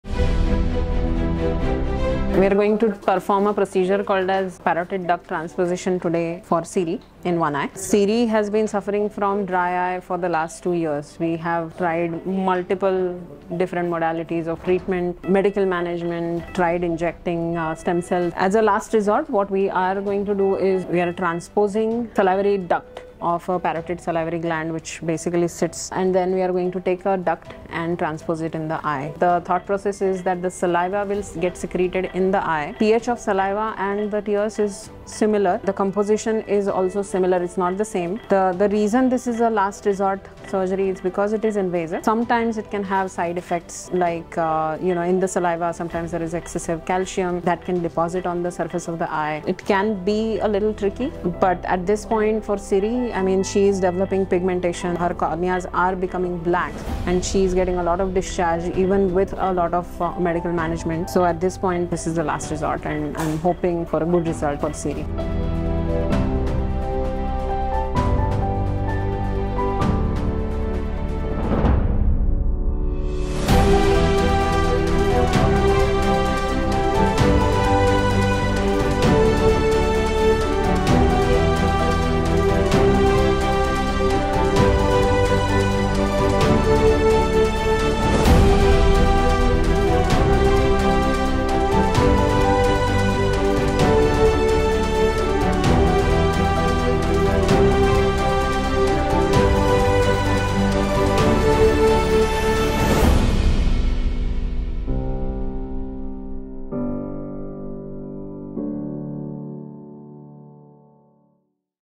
Can saliva save vision? - The Eye Vet - Episode 80
Siri, a sweet 6 yr old Lab was presented to us with sudden development of dry eye in both her eyes almost 2 years ago.
We tried all different types of medical treatments to bring back her tears but unfortunately her tear production remained zero.
Finally we decided to transpose a duct that carries saliva to the mouth from parotid salivary gland to her eye. This is a proven technique to treat tough cases. Despite the possible challenges associated with the procedure, Siri is doing wonderful and within just one month, her cornea is already looking clearer and shiny

Veterinary Ophthalmic Consultations & Surgeries | Veterinary Ophthalmology Practice & Training Institute